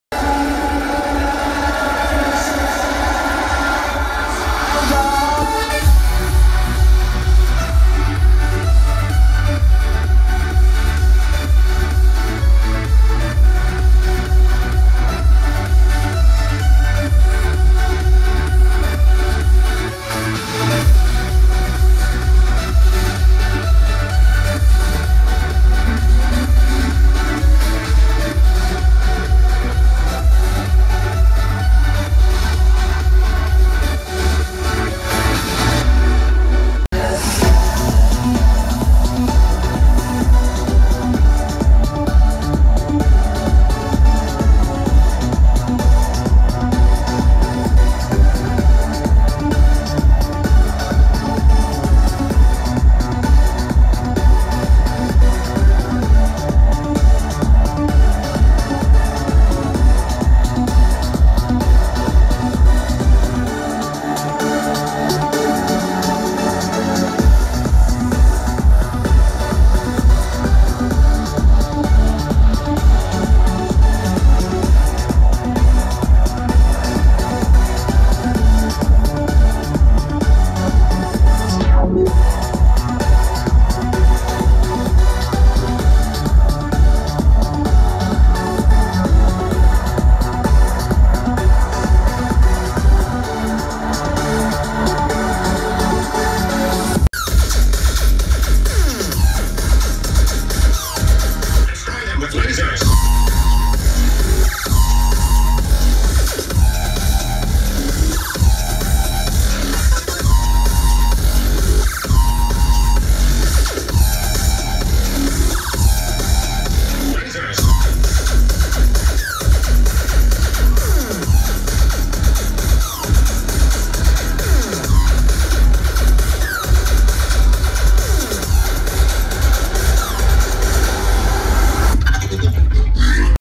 EMF in Guatemala with Dirty South, Zedd, and Skrillex, Erick Mejia and nu-salt laser
EMF in Guatemala with Dirty South, Zedd, and Skrillex nu-salt laser light shows international
2016
Erick Mejia, head production manager